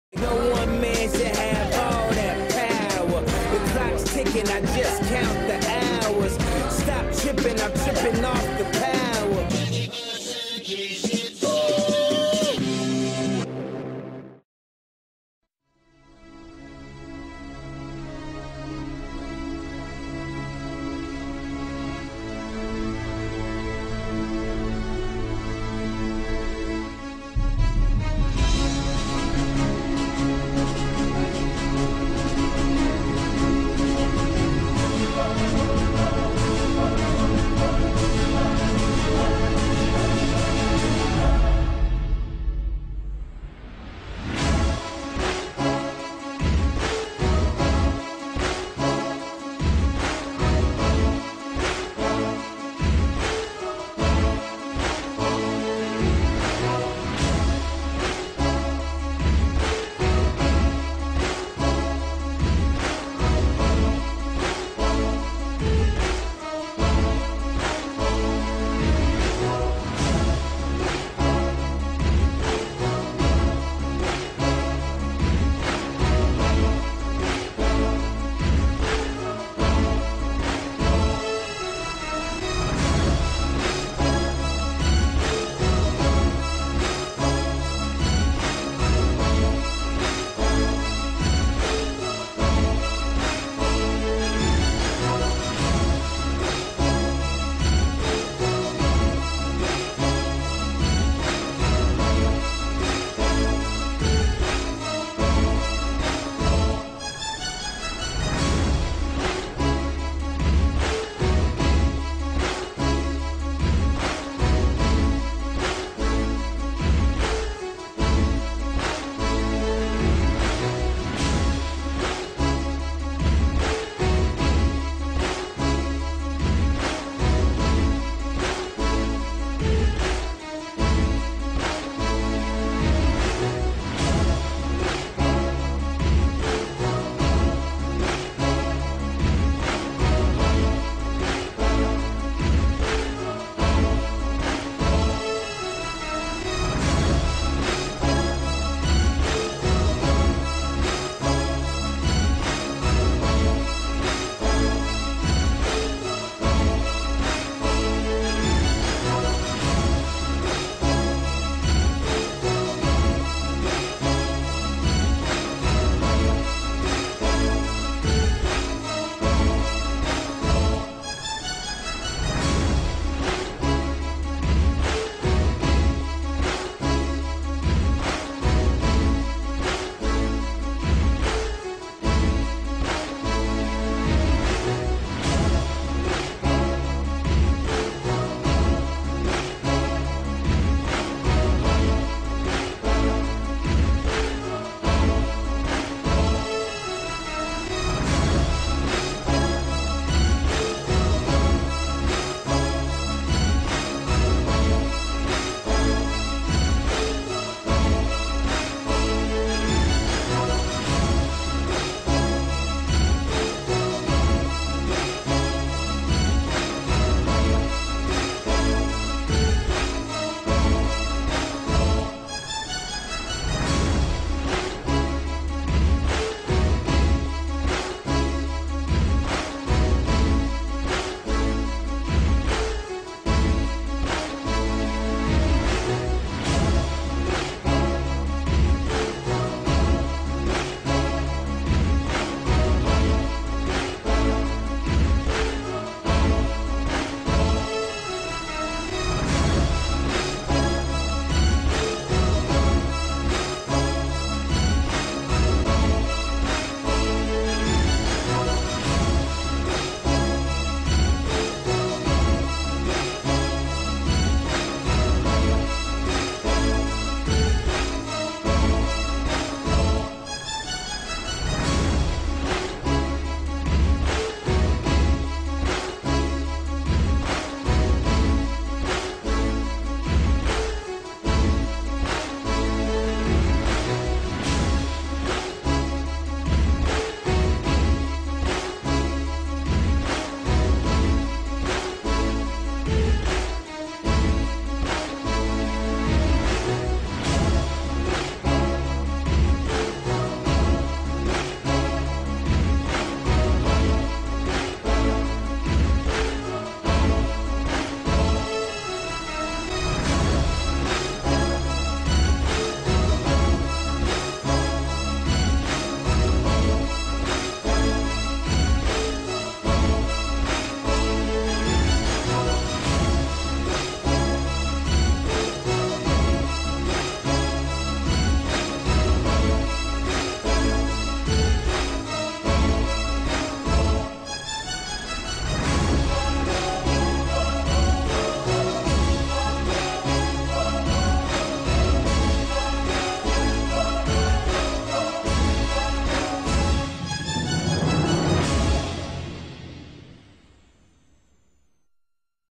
Jason Taylor career highlights
Miami Dolphins pass rusher Jason Taylor highlights. Sacks, interceptions, touchdowns from his illustrious career with Miami.